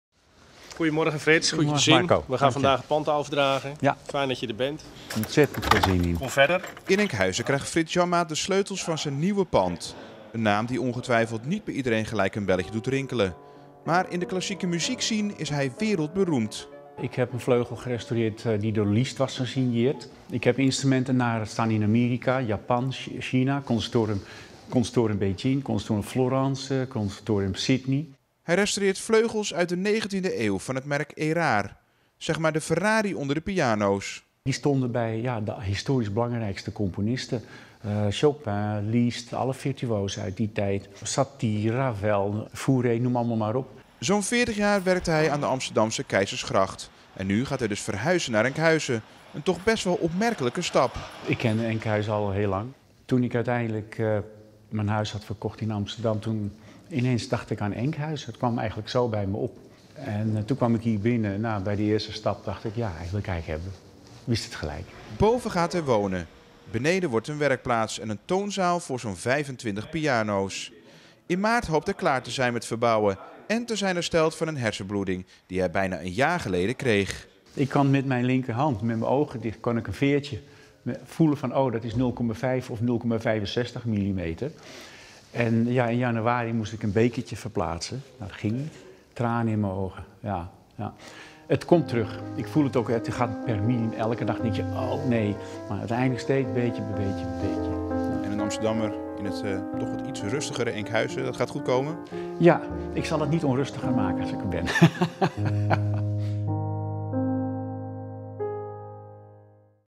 Wereldberoemde pianorestaurateur verhuist naar Enkhuizen
Janmaat is een begrip op het gebied van Erard-vleugels, de Ferrari onder de piano's.